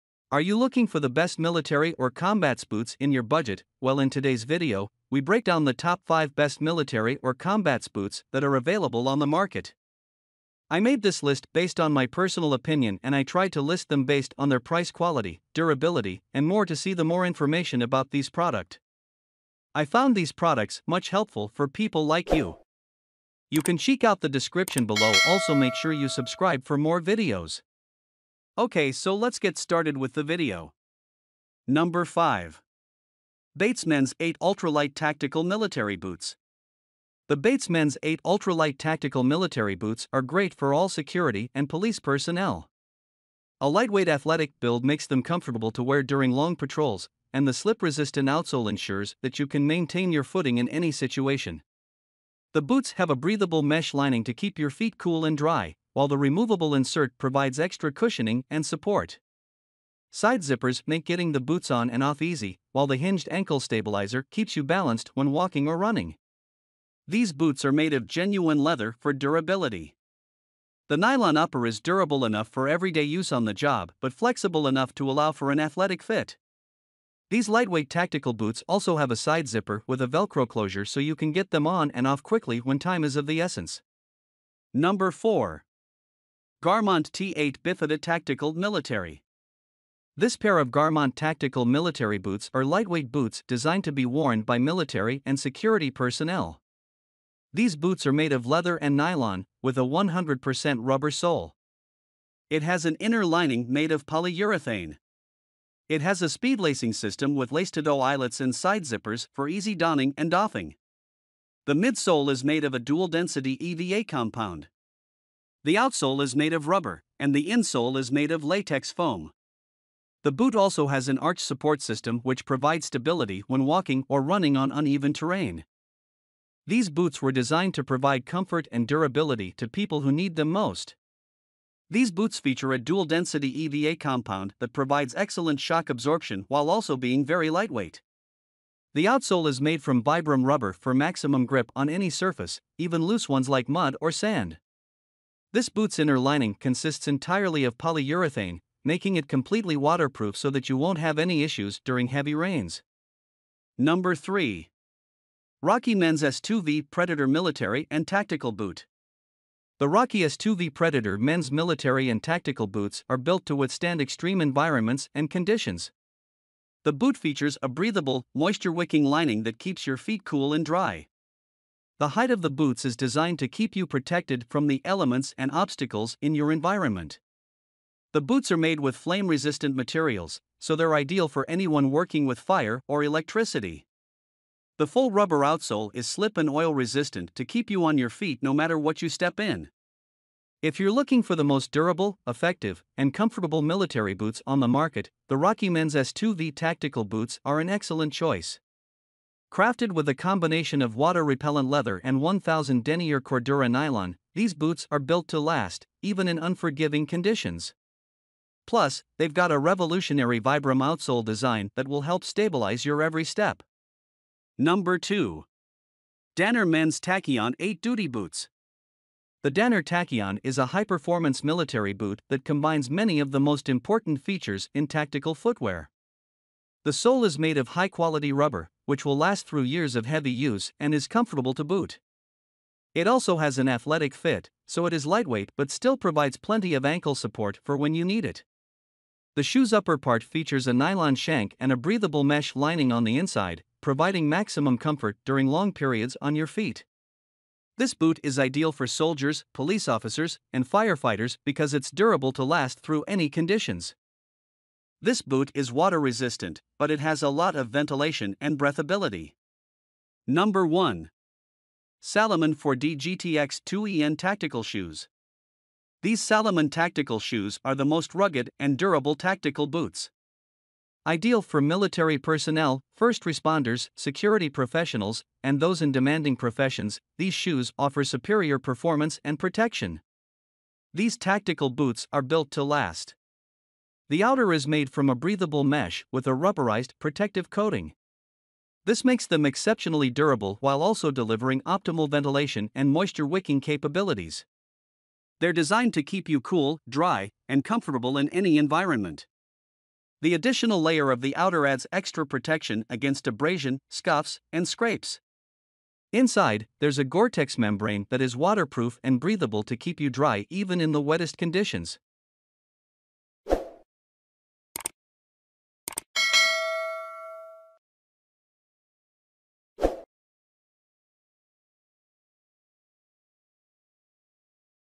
Best Military or Combats Boots - Top 5 Military Boots
In this video we are short listed the top Best Military or Combats Boots - Top 5 Military Boots on the market. We made this list based on our personal opinion.

5. Bates Men’s 8″ =
4. Garmont T8 Bifida =
3. Rocky Men’s S2v =
2. Danner Men’s Tachyon 8″ =
1.